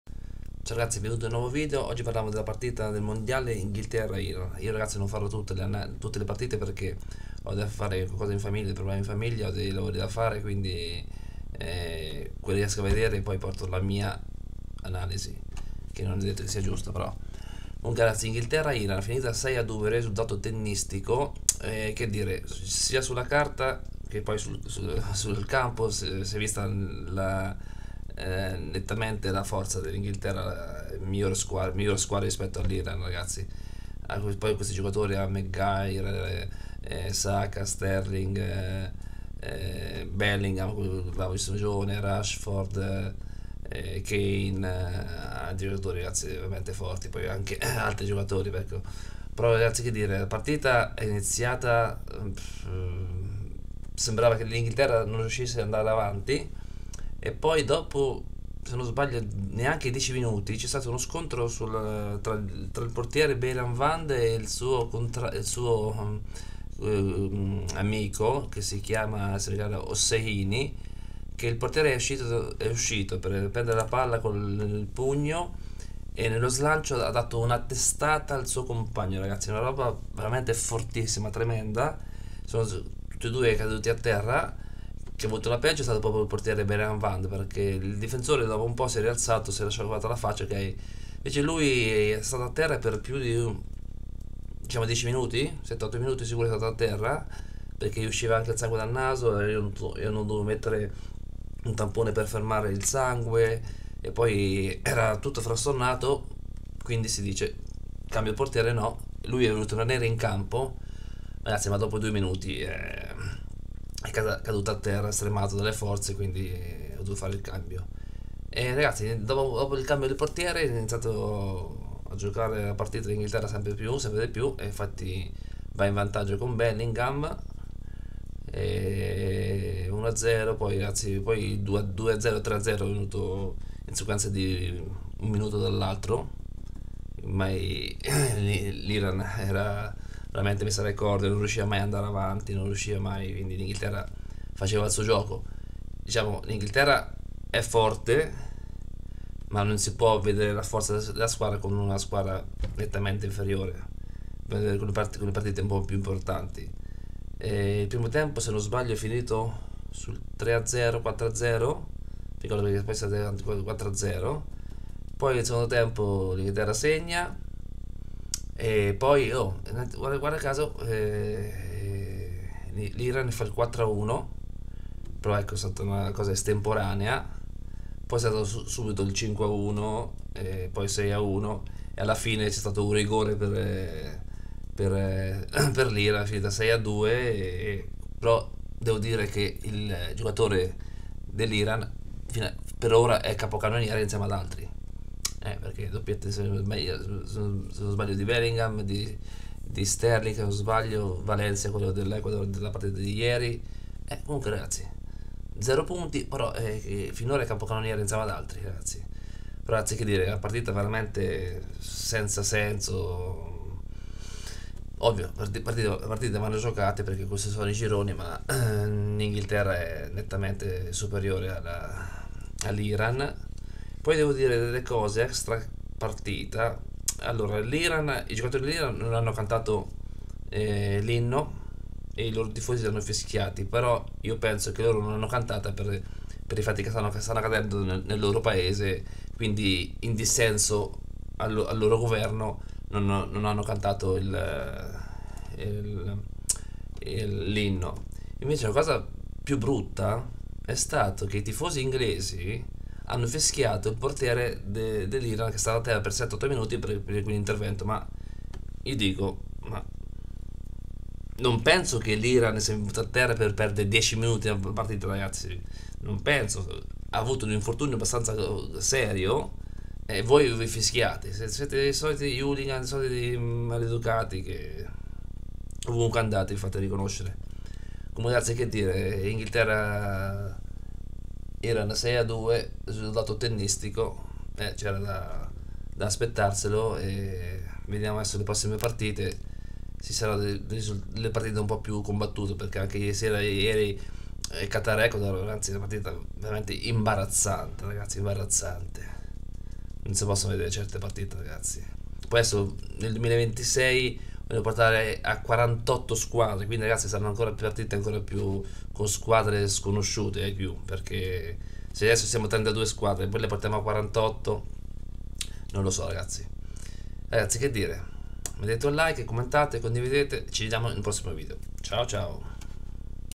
Inghilterra Iran 6-2... Risultato scontato QATAR 2022
L'Inghilterra si sbarazza con un risultato tennistico dell'Iran per 6-2